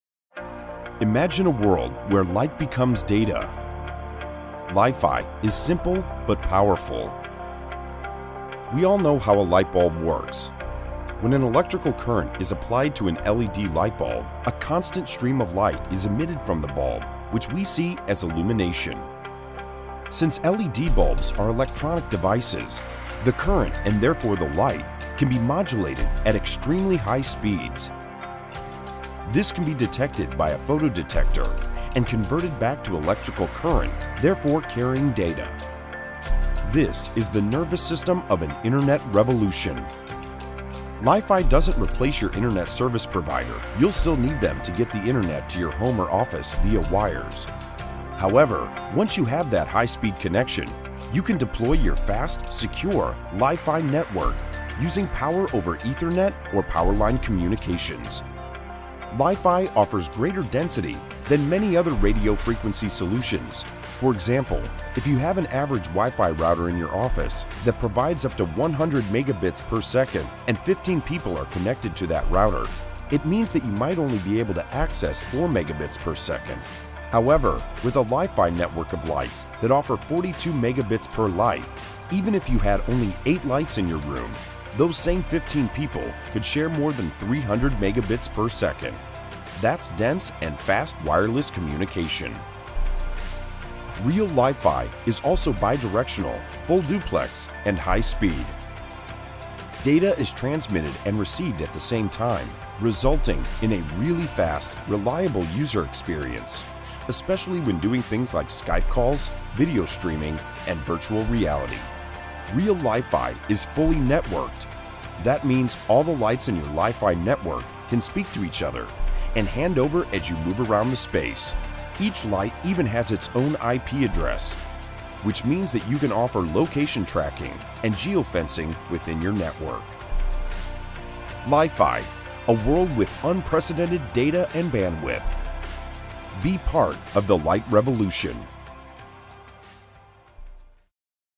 How Does LiFi Work?
Video from the leading Li-Fi company pureLiFi.

Li-Fi, also known as "Light Fidelity" is a wireless optical networking technology, which uses light-emitting diodes (LEDs) to transmit data. When an electrical current is applied to a LED light bulb a stream of light (photons) is emitted from the bulb. LED bulbs are semiconductor devices, which means that the brightness of the light flowing through them can be changed at extremely high speeds. This means that the signal can be sent by modulating the light at different rates. The signal can then be received by a detector which interprets the changes in light intensity (the signal) as data.  Also when the LED is ON, you transmit a digital 1 and when it is OFF you transmit a 0.  The intensity modulation cannot be seen by the human eye, and thus communication is just as seamless as other radio systems, allowing the users to be connected where there is LiFi enabled light. Using this technique, data can be transmitted from a LED light bulb and back at high speeds.